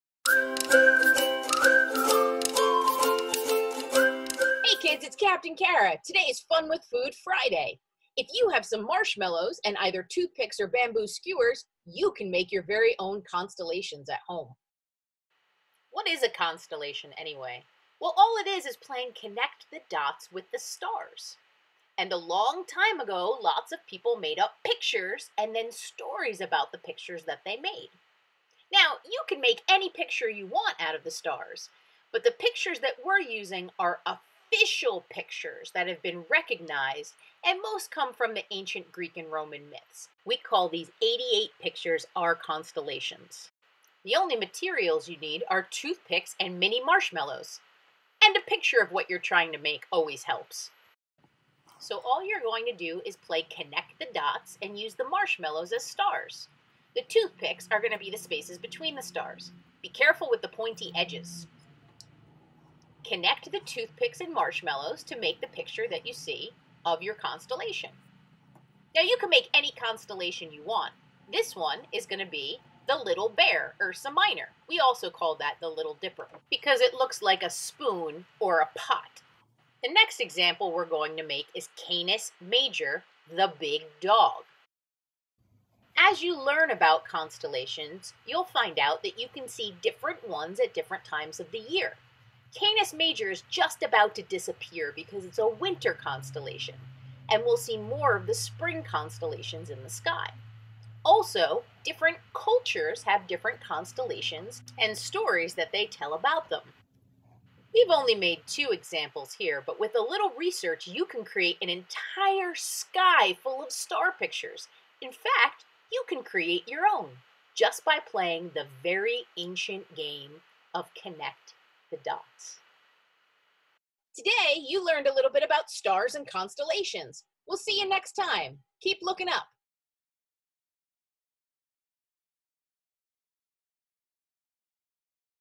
Marshmallow Constellations
We are making Marshmallow Constellations! What better way to have fun with your food!
We hope you have fun, learn, and of course, share your marshmallow constellations in the comments below!